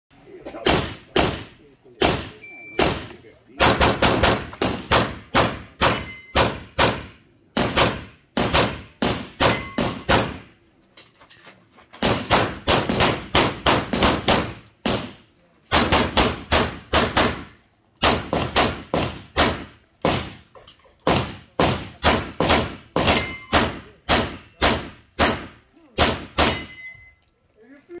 2^ Prova Campionato Federale Fitds
2^ prova di Campionato Federale
IV Trofeo Sardara Termale
ASD Terme di Sardara
Match Director: Garau Salvatore